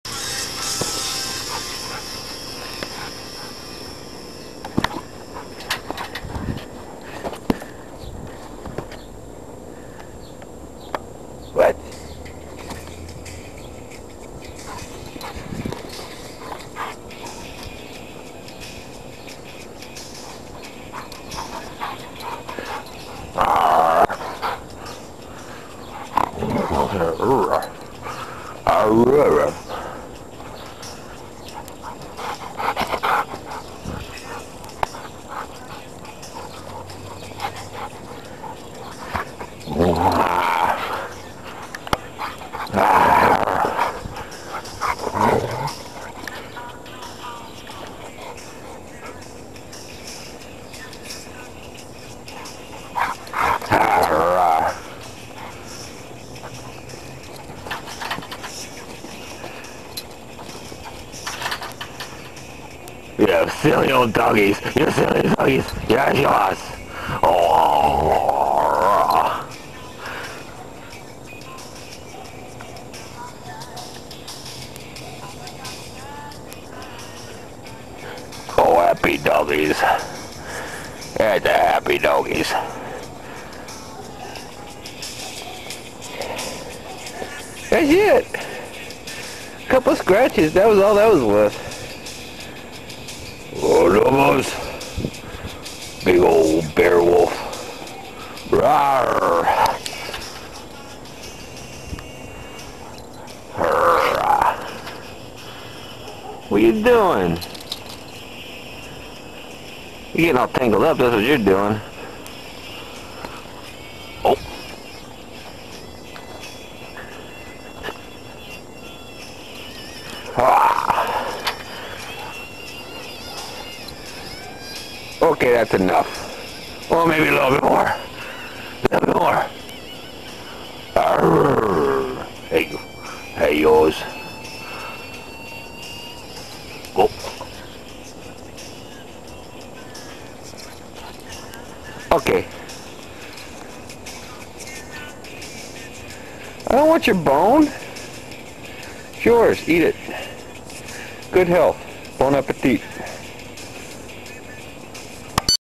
My Oldest Husky - Lobo - 5 30 09
Lobo is about 10 yrs old, he was a rescue dog so we really aren't sure... He is still active though, of that there is no doubt.  X^)